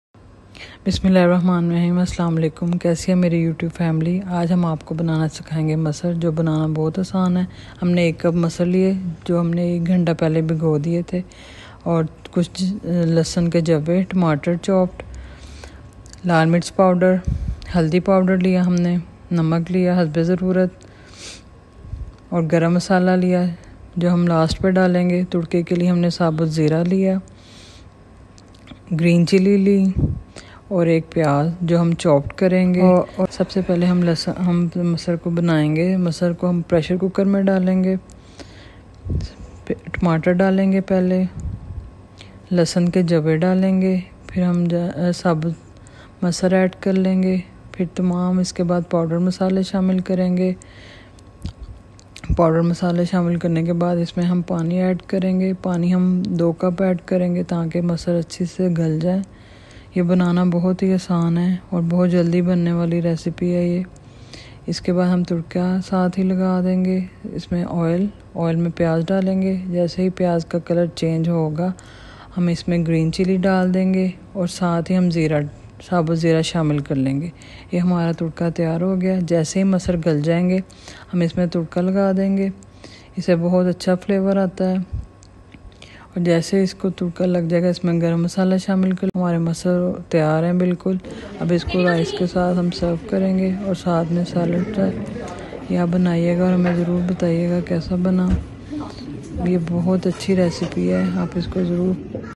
How to make Sabot Masar| Easy to make masar dal| Sam's kitchen| delicious dal ka salan
We share old recipes with new tarka😘,In this video we share yuummy recipes of food🙂😘try it.